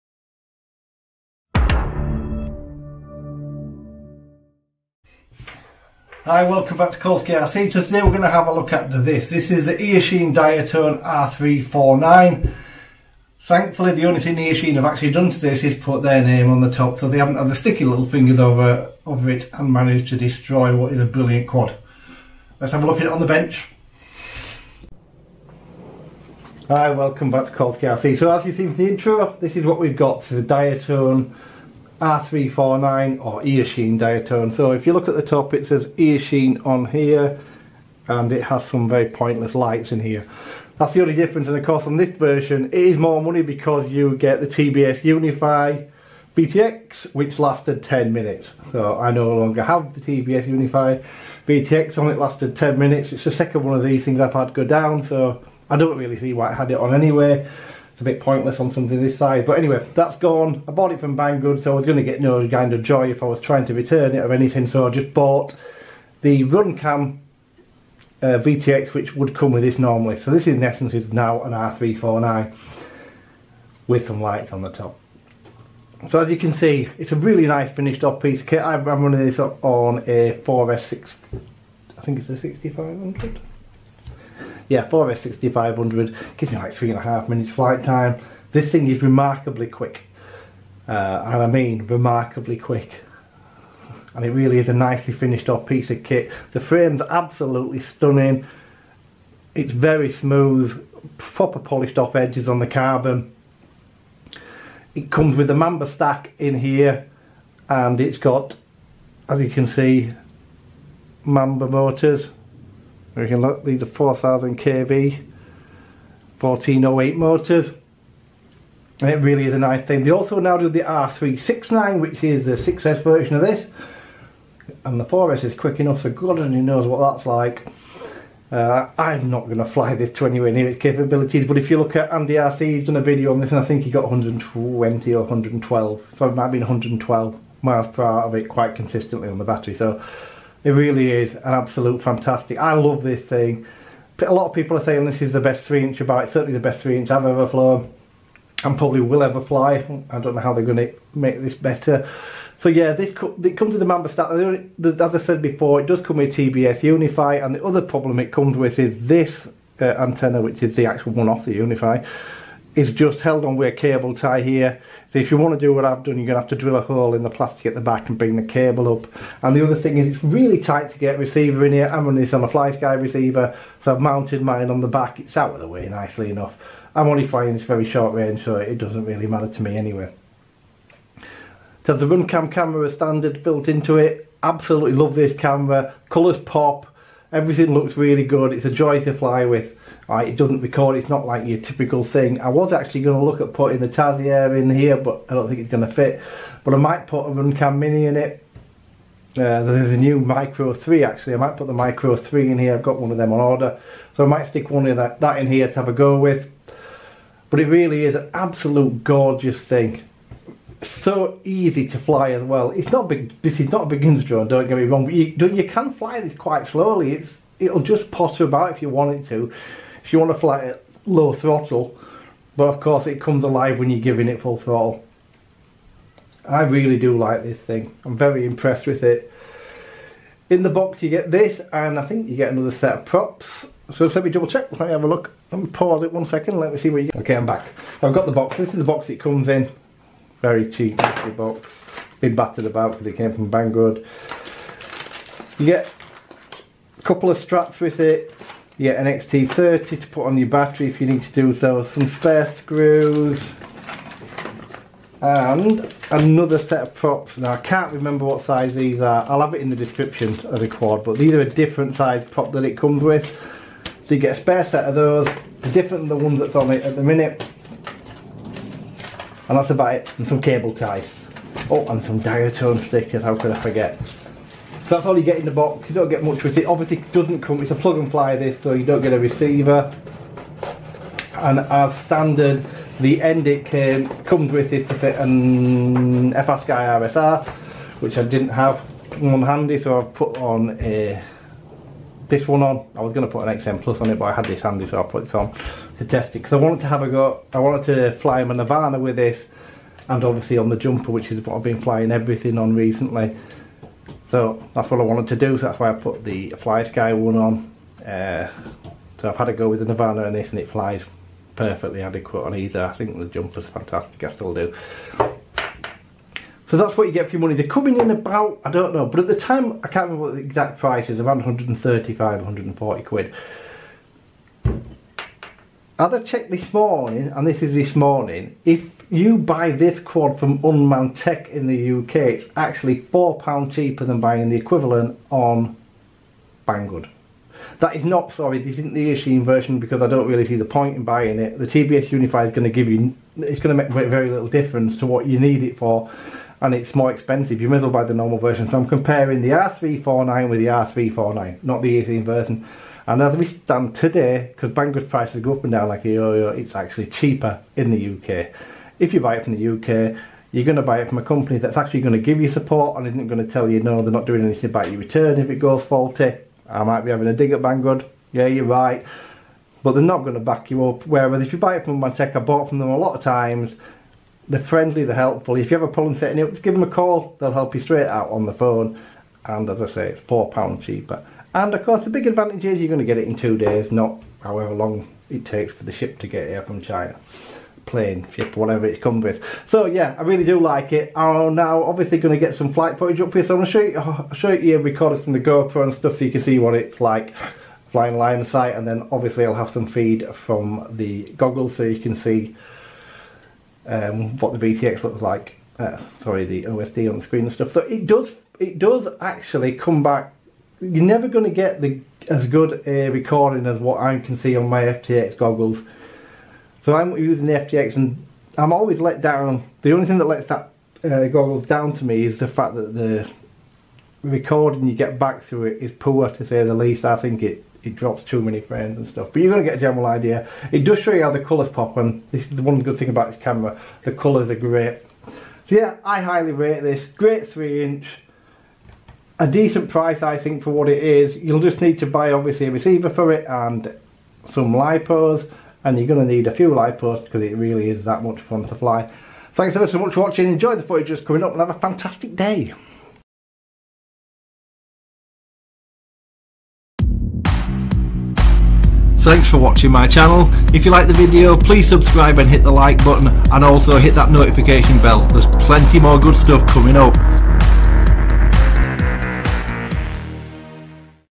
EACHINE AND DIATONE ER349 IT'S AWESOME
Review of the Diatone and Eachine ER349

All my reviews are 100% honest and do not contain edited flight footage everything you see is what the drone produces, no colour grading or stabilisation has been added.

Full specs below:

Brand Name: EACHINE&DIATONE
Item Name:  ER349
Lipo: 4S
Props: 3 inch
Wheelbase: 135mm
Weight: 140g
Camera:  RunCam Micro Swift
Antenna: Copper pipe antenna
VTX: TBS UNIFY 800mW
FC: MAMBA F405 MINI MK2
ESC: MAMBA 254 /25A 4S ESC Dshot 1200
Motor: MAMBA Racing MB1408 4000KV
Click For Recommend Parts.

Spare parts specifications:
FC: MAMBA F405 MINI MK2(Limited Edition)
MCU: 168MHz STM32F405RGT6
IMU: MPU6000
OSD: AT7456E
BlackBox: 16M Flash Memory
Soft: Dshot 150/300/600/1200
Mounting: 20mm/M2
Weight: 10.5g
Camera Control: YES
Smart Audio: YES
RSSI: YES
LED Controller: BF * 1Set
Power
BEC: 5V 1.5A
Input: 2~4S (8.4V~16.8V)
Current: 25A*4Set
Burst: 30A (10S)
CURR: YES

Please Note:
The PNP (Plug and Play) version comes without battery, charger, receiver or remote control.

Package included:
1x EACHINE&DIATONE ER349 3 inch 4S PNP FPV Racing Drone